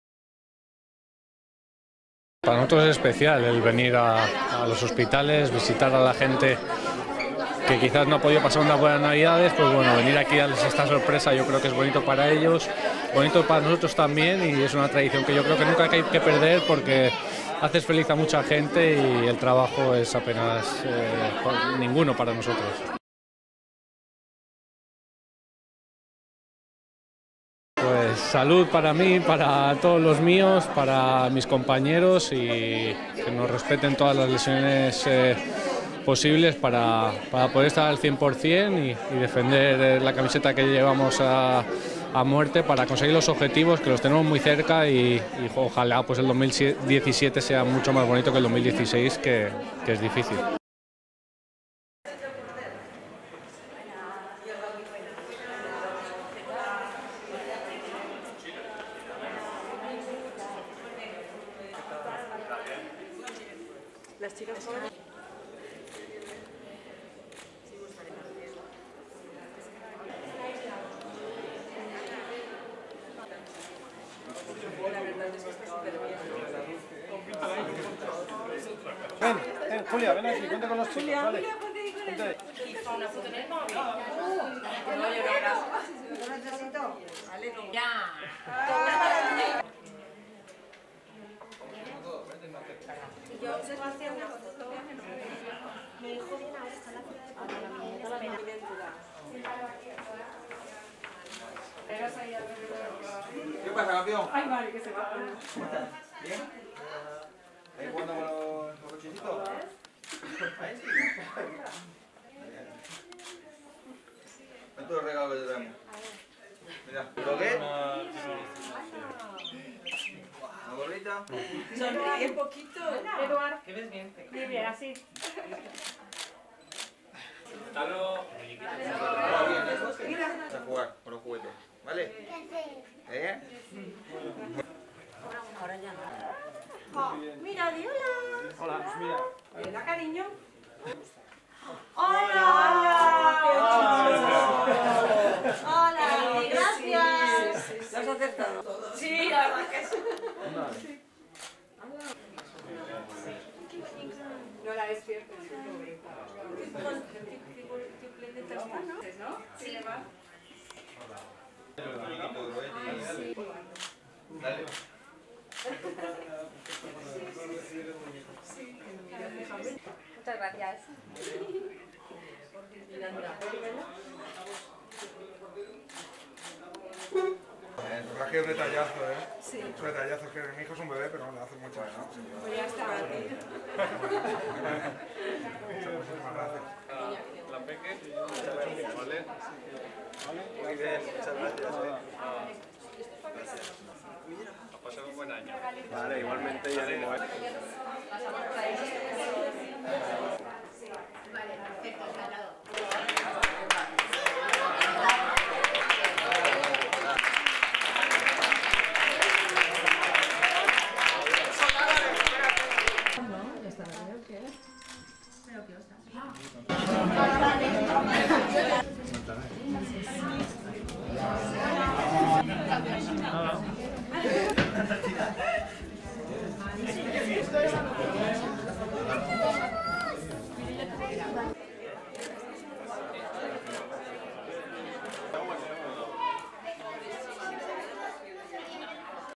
El Villarreal CF visita diversos hospitales
El Villarreal CF ha visitado en la jornada de hoy diversos hospitales. Imágenes de la visita al Hospital de La Plana (Villarreal) con declaraciones de Roberto Soldado.